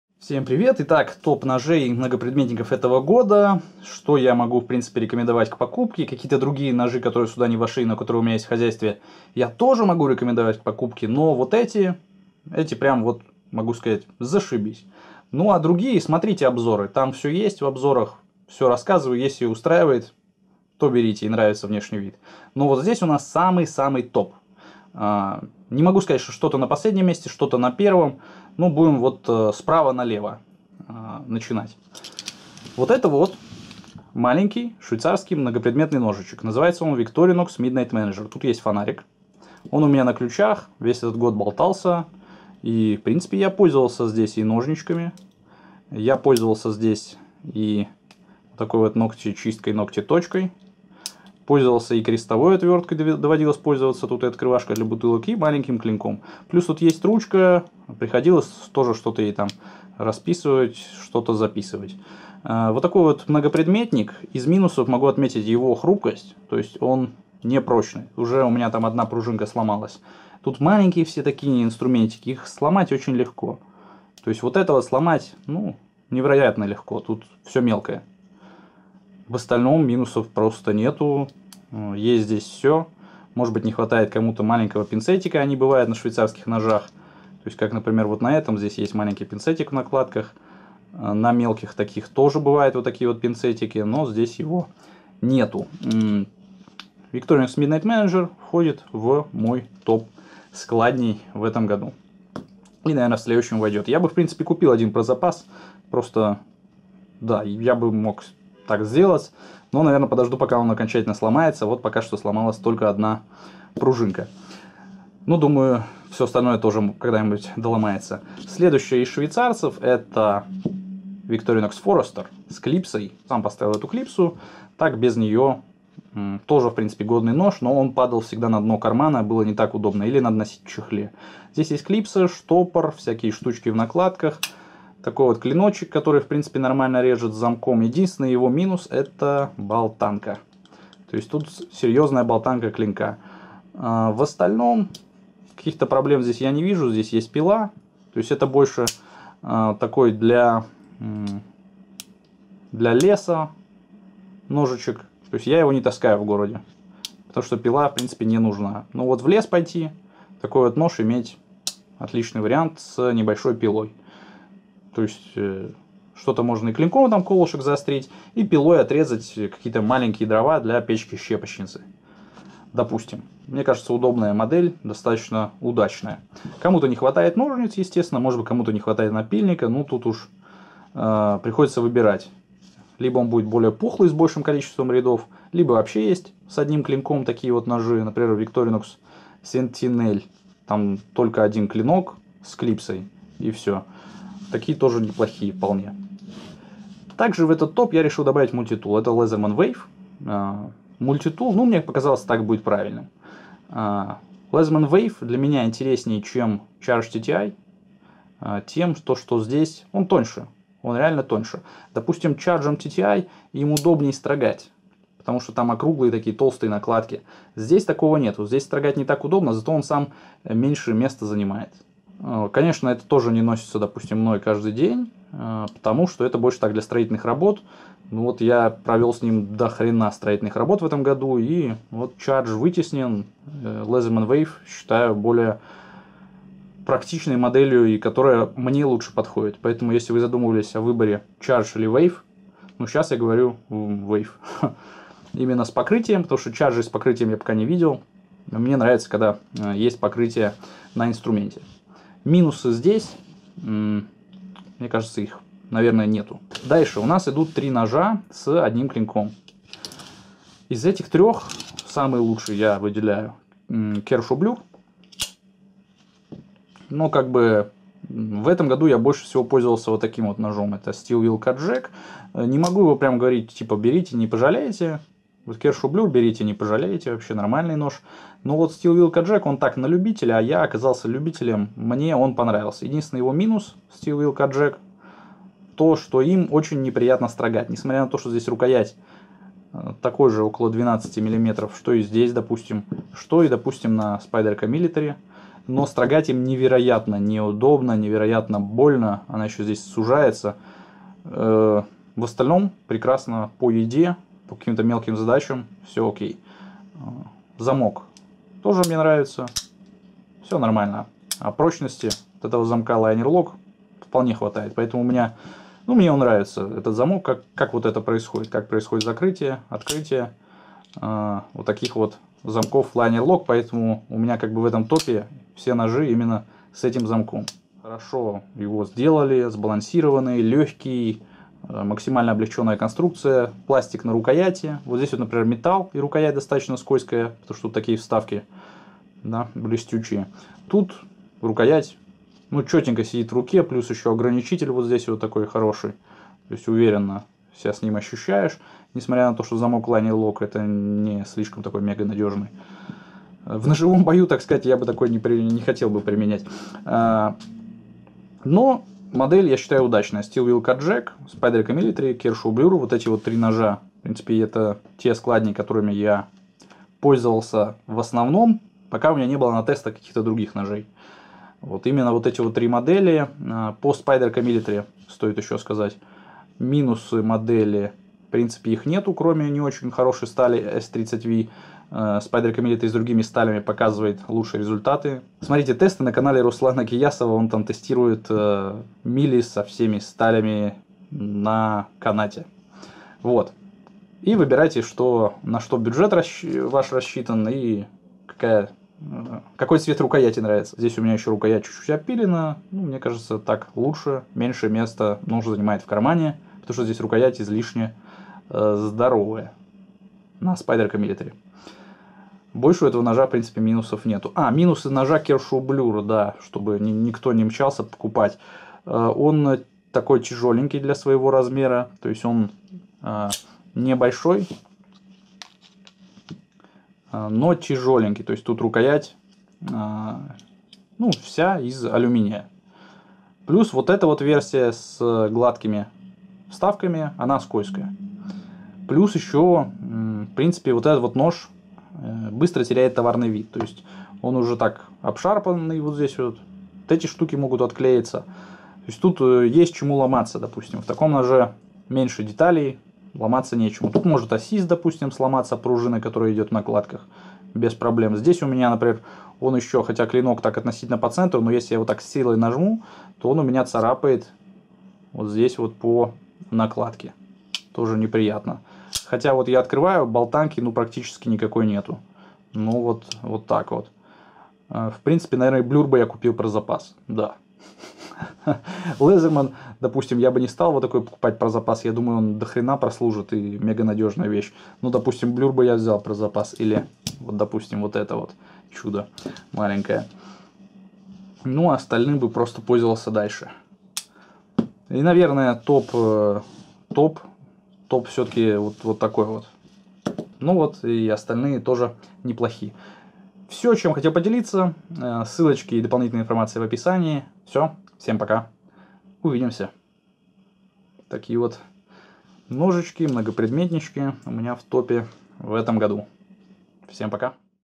Ножи года по версии Tactical+ / Топ моих ножей 2018-го года
Данные складные ножи / многопредметники я использовал в течении года (когда не тестировал специально другие модели) и вот что я хочу сказать.

В кадре были:
6. Victorinox Midnite Manager 0.6366
5. Викторинокс Forester One hand 0.8361.MC + clip
4. Мультитул Leatherman Wave Plus
3. Spyderco Military SC36GPCMO
2. Steel Will Cutjack C22-1OD
1. Kershaw Blur 1670SG10

Но в принципе другие складни, что я показывал на канале в этом году, тоже норм. Но они не однозначно зашибись. А в целом продолжает действовать парадокс ножемана (чем дольше пользуешься - тем больше нравится конкретный нож)